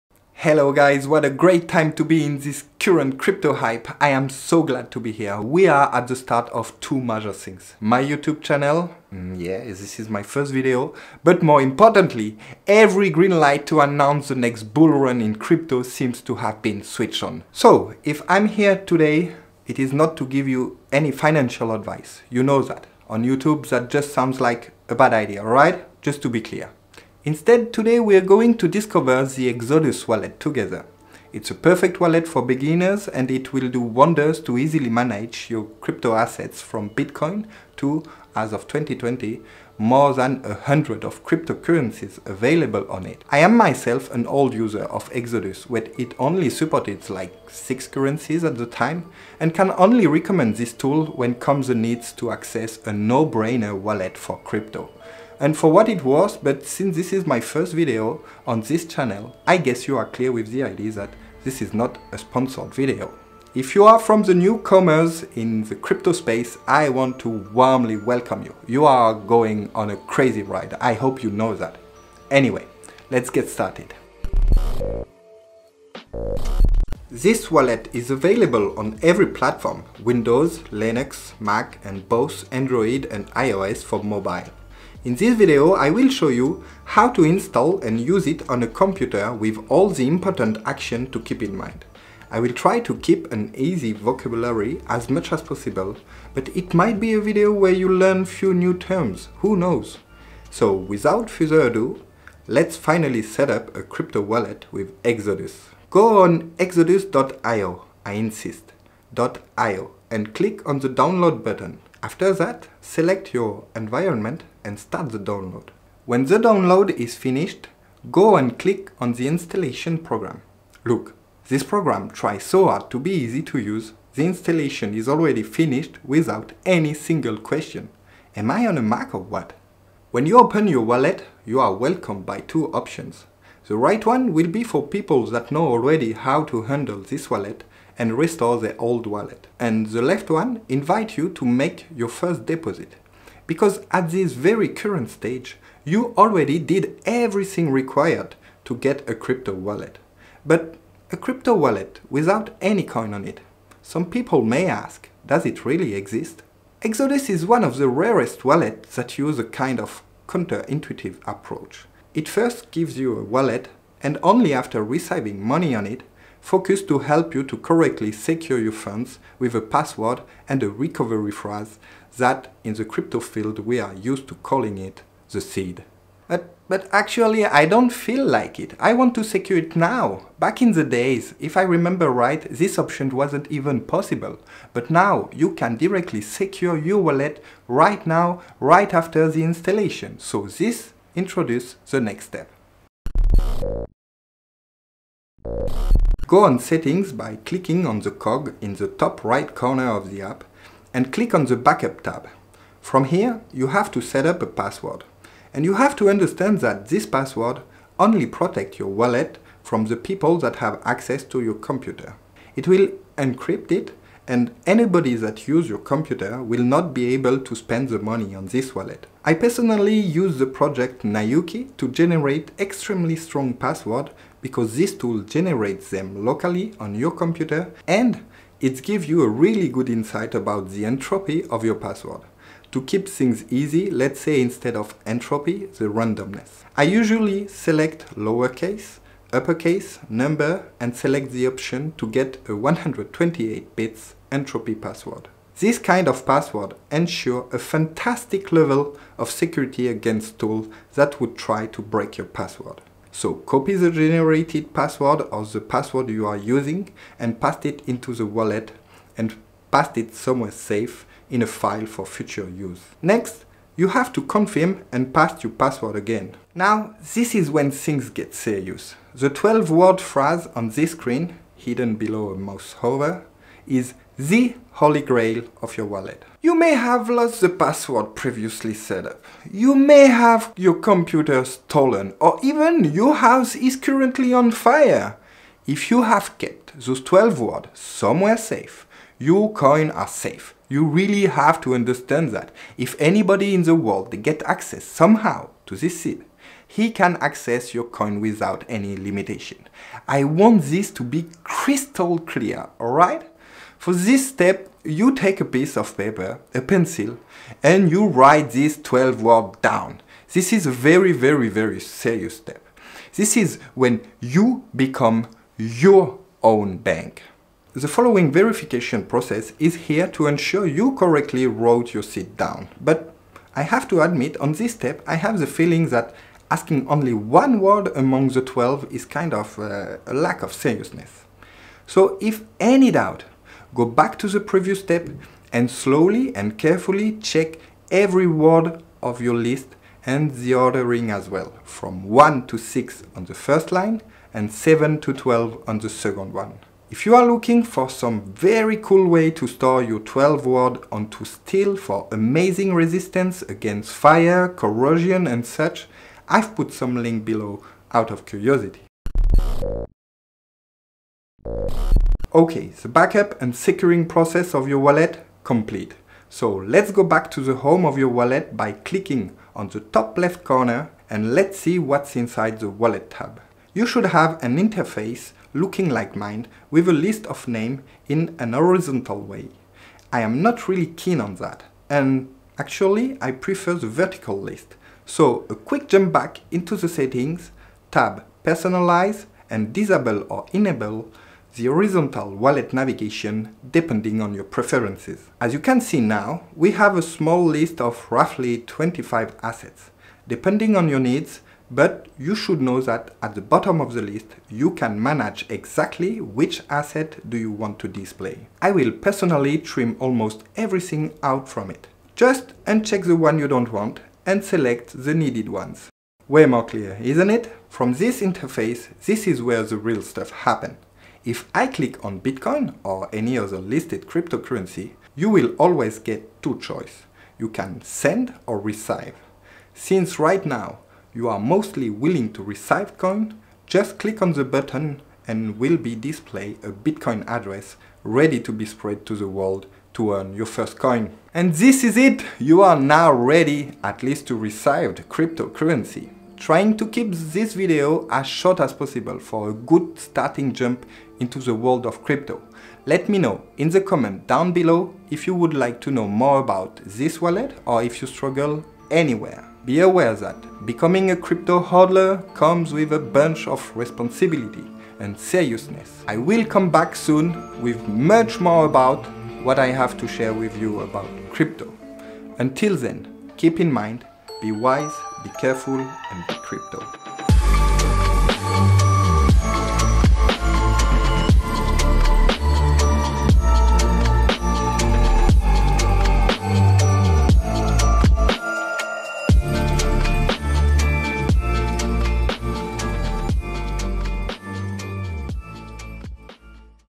💰 Easiest Crypto Wallet 2021 with Exodus
How to easily receive cryptocurrency explained. Use the Exodus Wallet to receive more than a 100 different currencies.

This video cover :

Installing the wallet exodus
Securing and backup the wallet
Overview how to receive bitcoin with Exodus

0:00 Presentation
1:13 Installation
3:13 Securing your wallet
6:00 Wallet overview